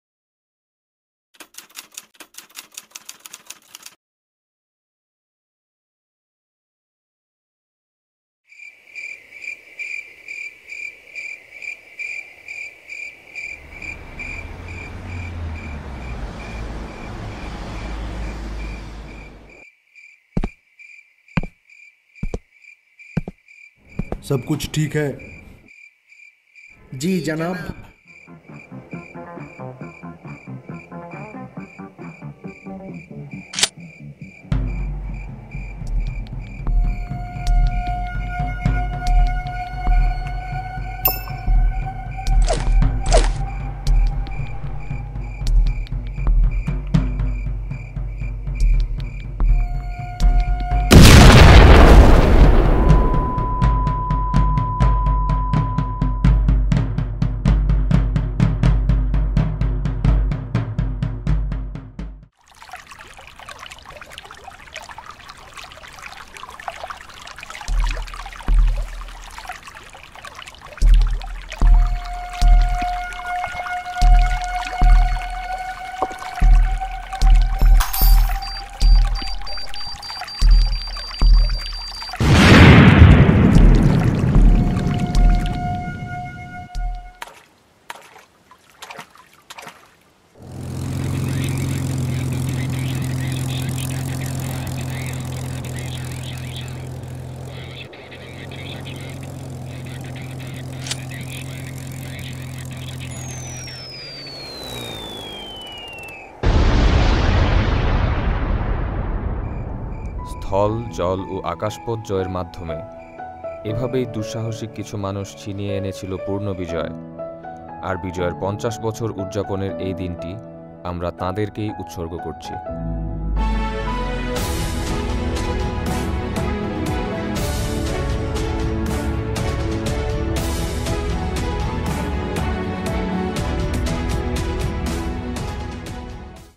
Happy 50th Victory Day!
পরাধীনতার শেকল থেকে দেশকে মুক্ত করতে জীবন বাজি রেখে লড়েছেন যাঁরা, আজ, বিজয়ের এই সুবর্ণ জয়ন্তী-তে আমরা বিনম্র শ্রদ্ধায় স্মরণ করছি তাঁদের।
সবাইকে ৫০-তম বিজয় দিবসের শুভেচ্ছা।
On the Golden Jubilee of our Victory Day, we are respectfully recalling all those people who did not fear death and bravely fought for this country!